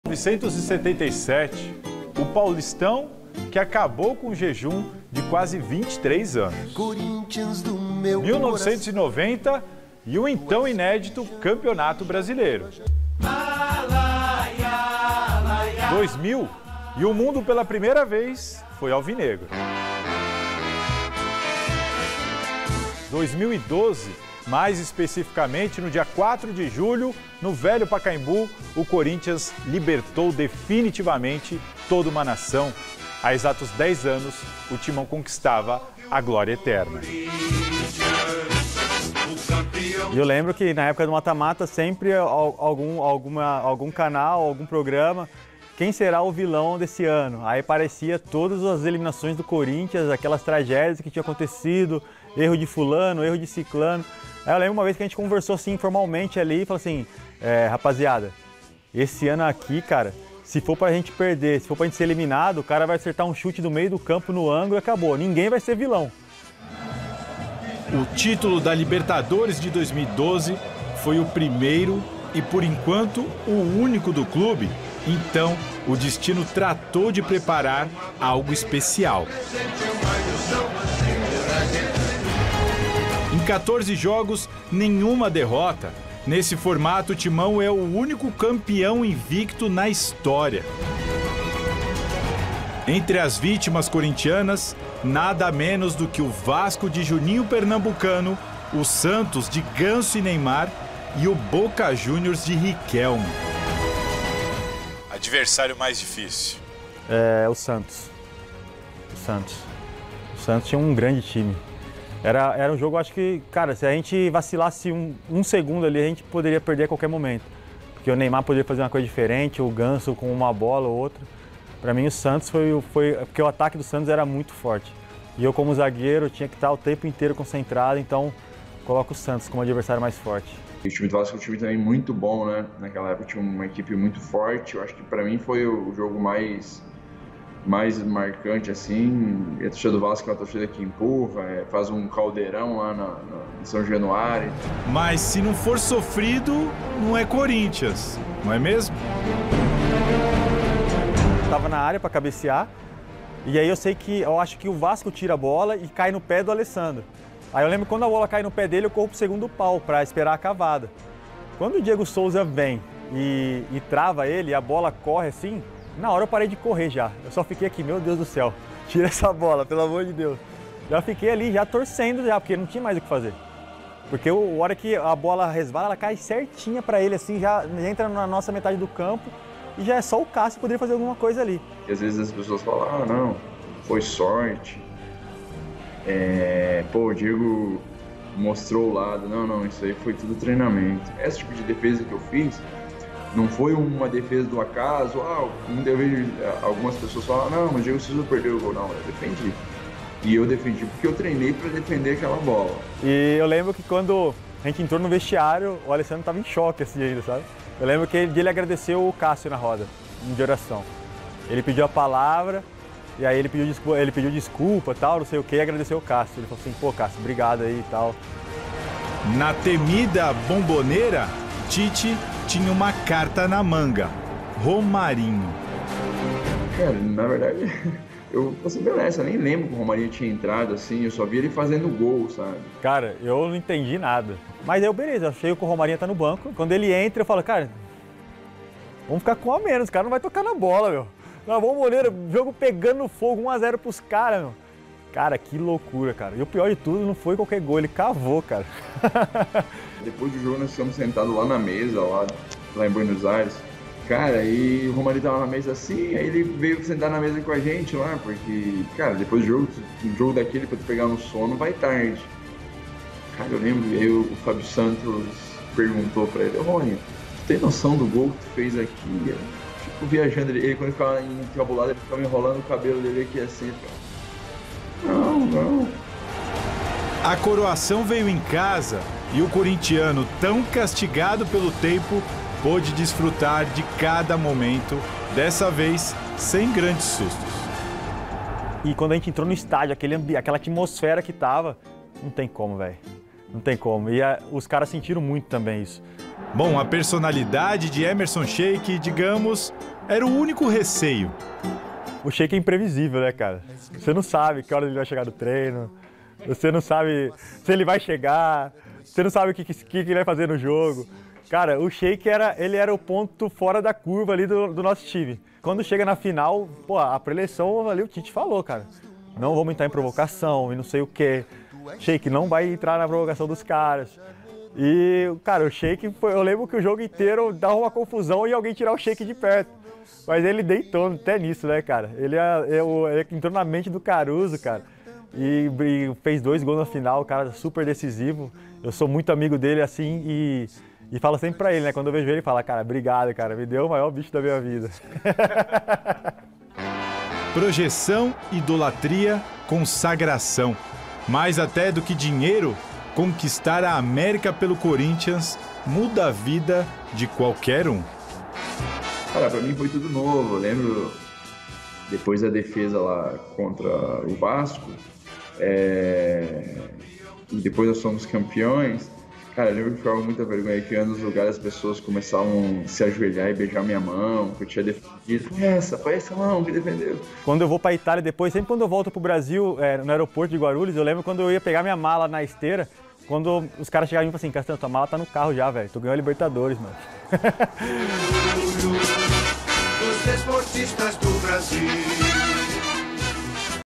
"Eu ia nos lugares e as pessoas se AJOELHAVAM pra mim", revela Cássio (03/07/22)
Único campeão INVICTO! Há exatos 10 anos, o Corinthians conquistou o título INÉDITO da Copa Libertadores contra o Boca Juniors, com dois gols de Emerson Sheik. Veja matéria especial do repórter Tiago Salazar com entrevistas exclusivas de Cássio e Leandro Castán.